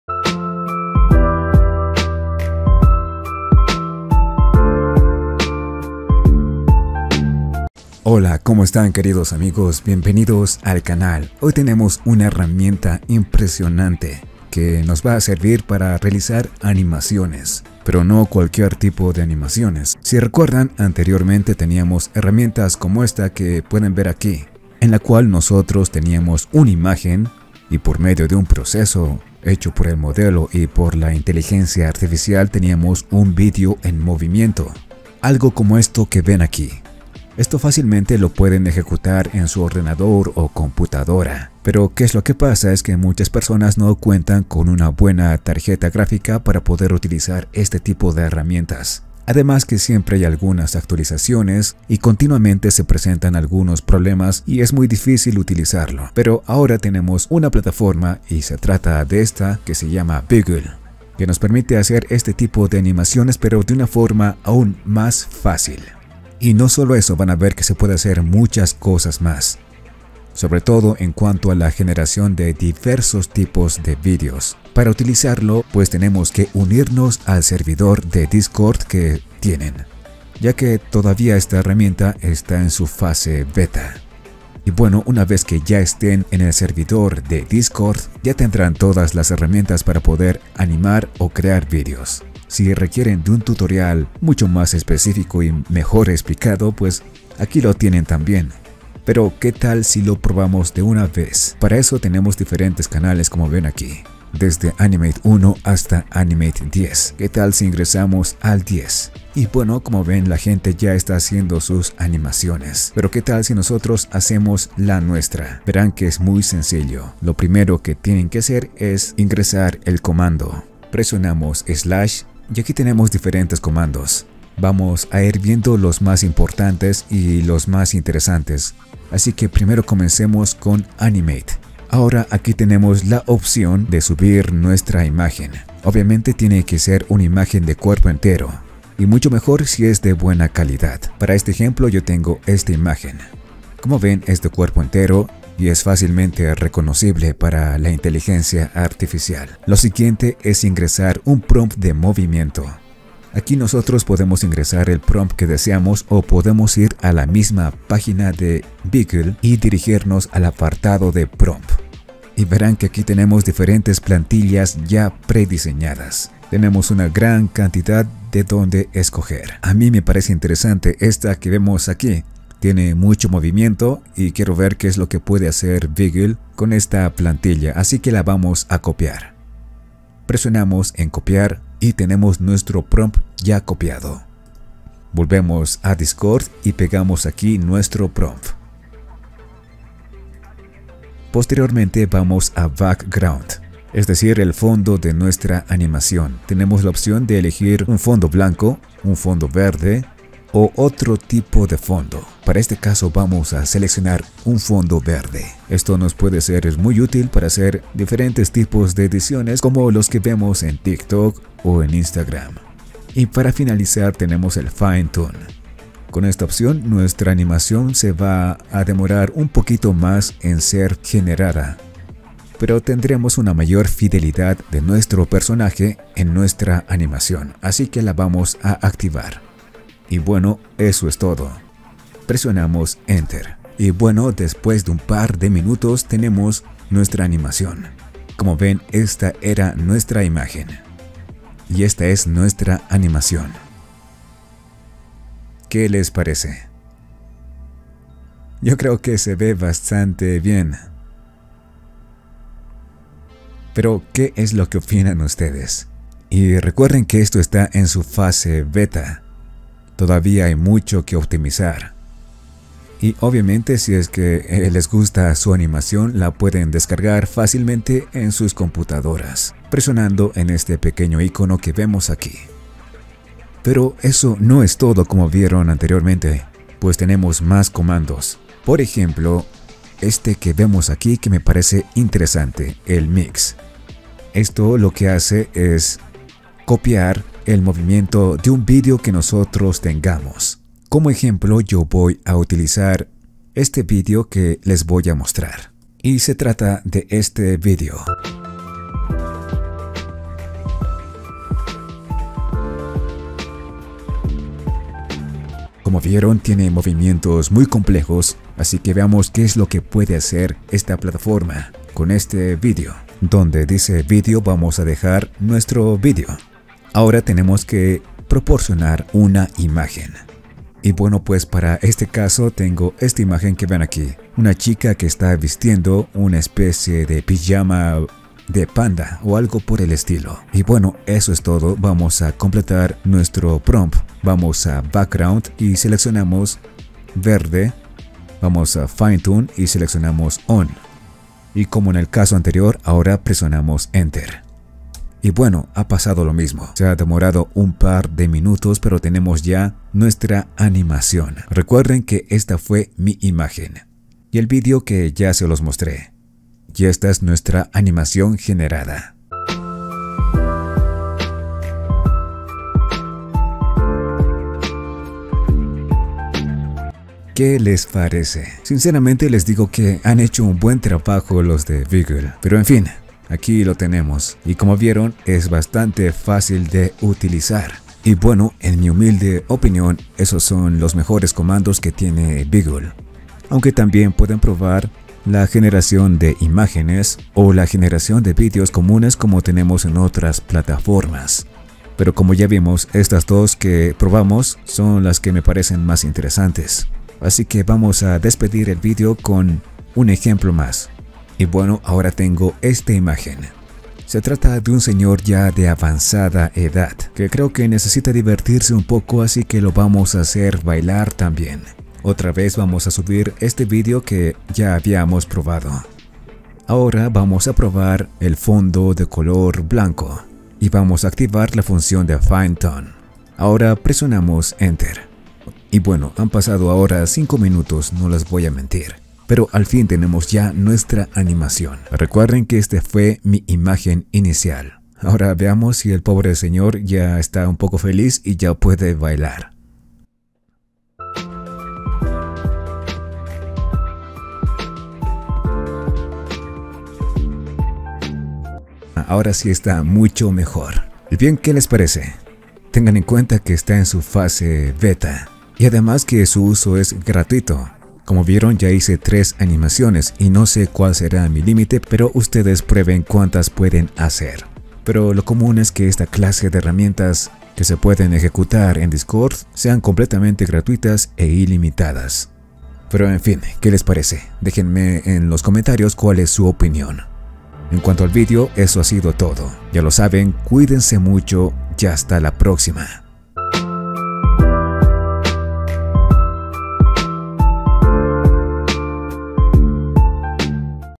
ESTE ES EL FUTURO DE LA ANIMACIÓN CON INTELIGENCIA ARTIFICIAL
Nueva IA para generar video, animar imágenes, crear imágenes y más, aprovecha a probarlo que es gratis!!

🔗 Enlaces

Si te gusto el video , considera apoyar al canal